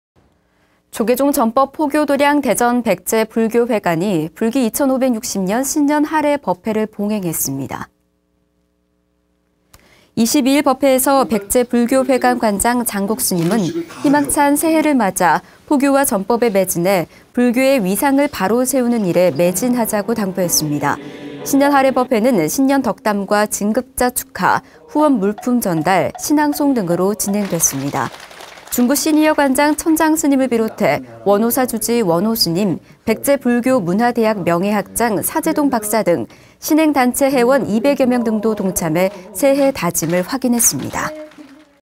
[BTN뉴스]백제불교회관 신년하례..불교 위상 높이는 한 해
조계종 전법포교도량 대전 백제불교회관이 불기2560년 신년하례법회를 봉행했습니다. 22일 법회에서 백제불교회관 관장 장곡스님은 “희망찬 새해를.. BTN불교TV | 01-25